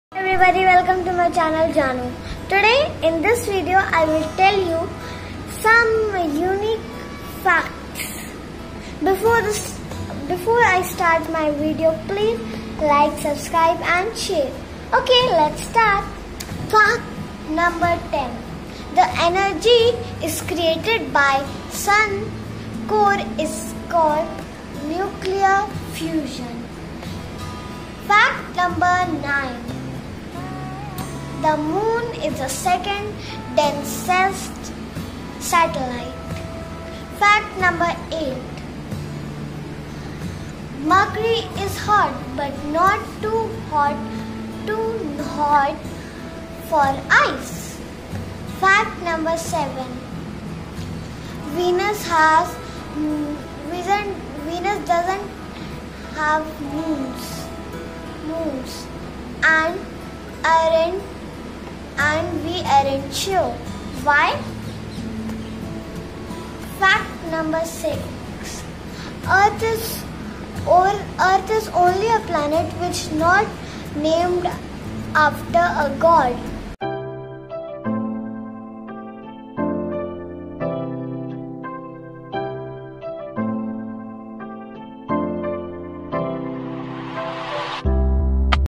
Some Unique Facts about Planets
Know some unique facts about planets like which planet is not in the name of any God ?